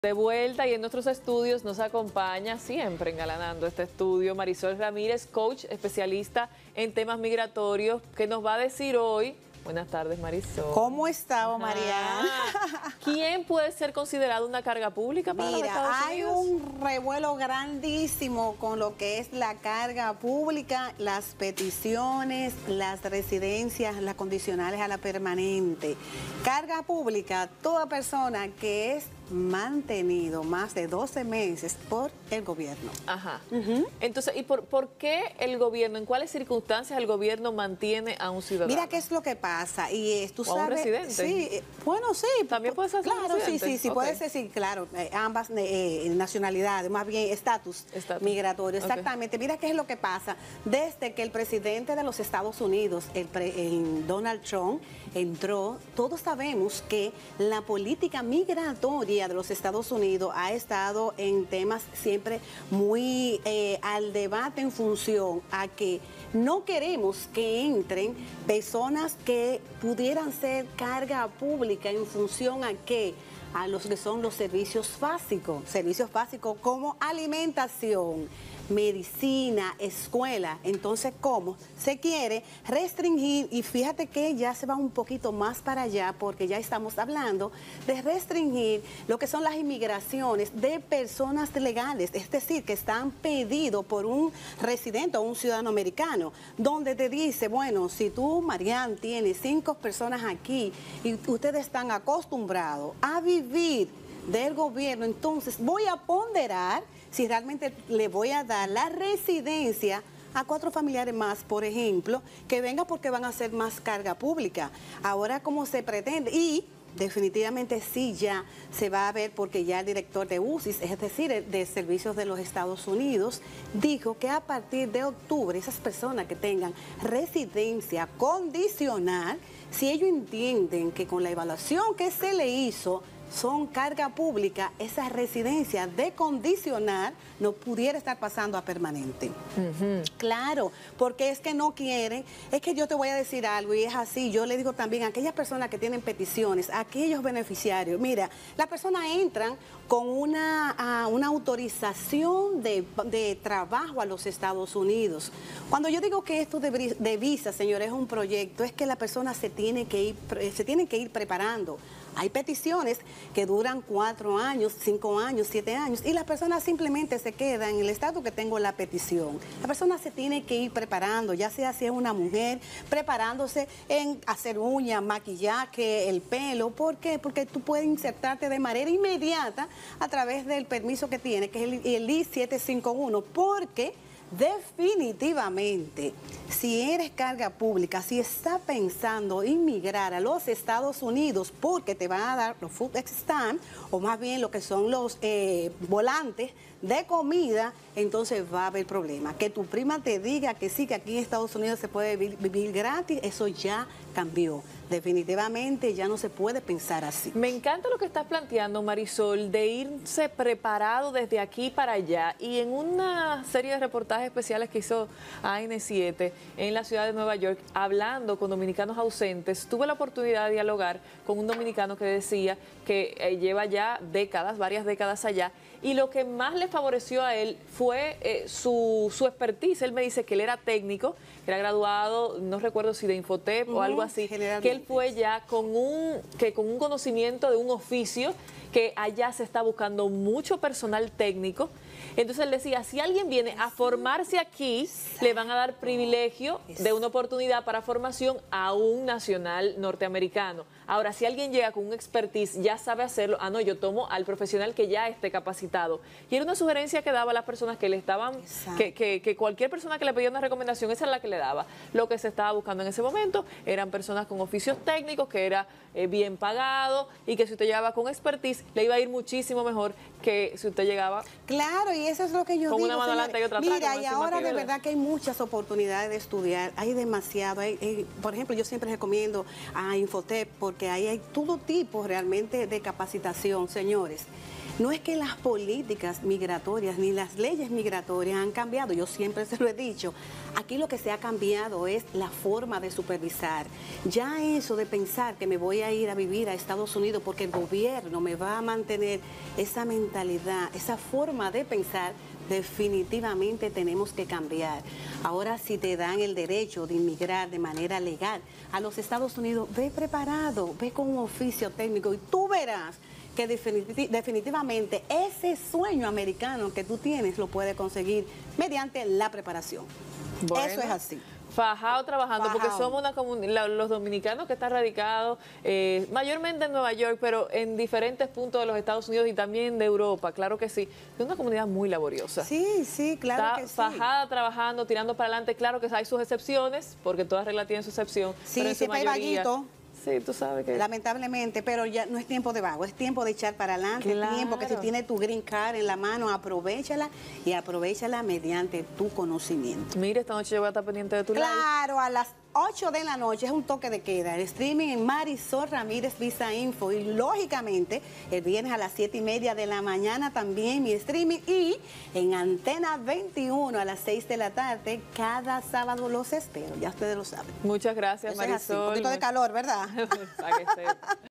Marisol Ramírez llega para responder todas las dudas en materia migratoria
¿Quiénes son considerados una carga pública para los Estados Unidos?

La aboga y experta en temas migratorios, nos aclara las nuevas medidas interpuestas por las autoridades estadounidenses.